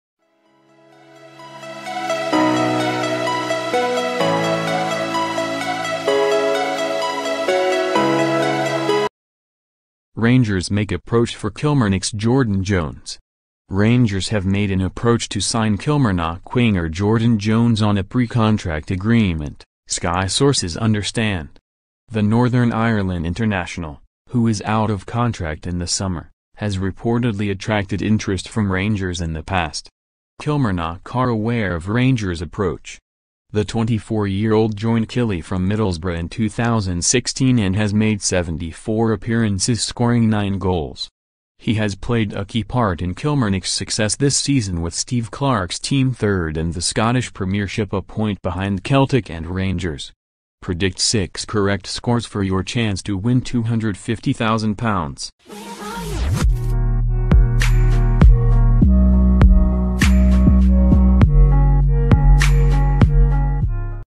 Rangers make approach for Kilmarnock's Jordan Jones
Rangers make approach for Kilmarnock's Jordan Jones. Rangers have made an approach to sign Kilmarnock winger Jordan Jones on a pre-contract agreement, Sky sources understand.

The Northern Ireland international, who is out of contract in the summer, has reportedly attracted interest from Rangers in the past.

Kilmarnock are aware of Rangers approach.

The 24-year old joined Killie from Middlesbrough in 2016 and has made 74 appearances scoring nine goals.

He has played a key part in Kilmarnock's success this season with Steve Clarke's team third in the Scottish Premiership a point behind Celtic and Rangers.